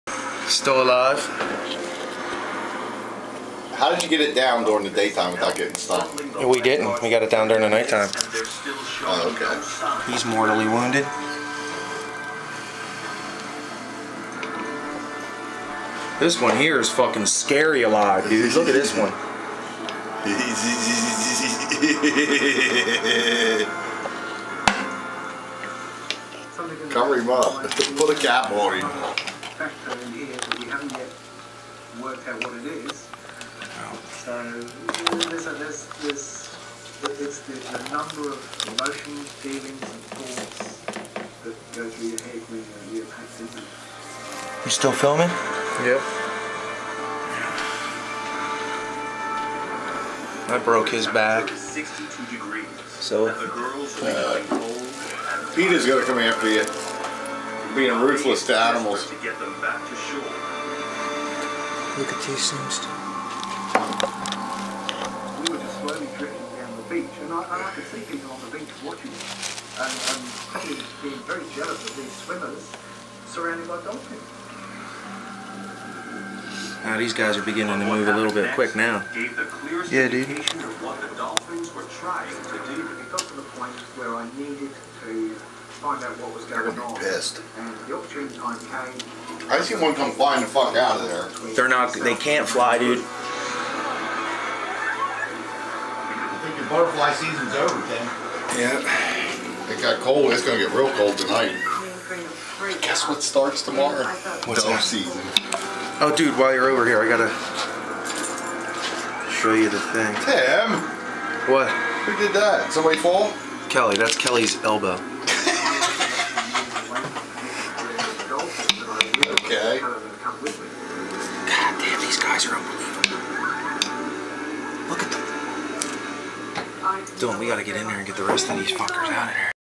Bald Faced Hornet Hive dissected
Bald Faced Hornet hive dissected while hornets are still alive!